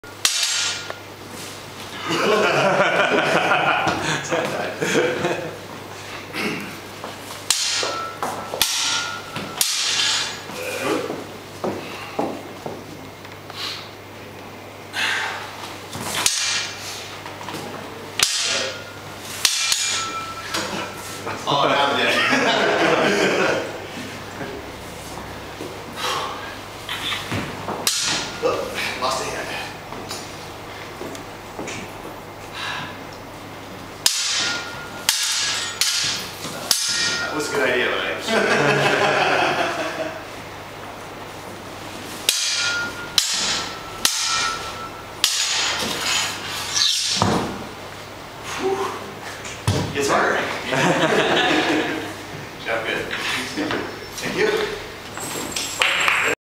Jeffrey Steefel in a sword fight
What you have to go through, if you are Executive Producer of Lord of the Rings online.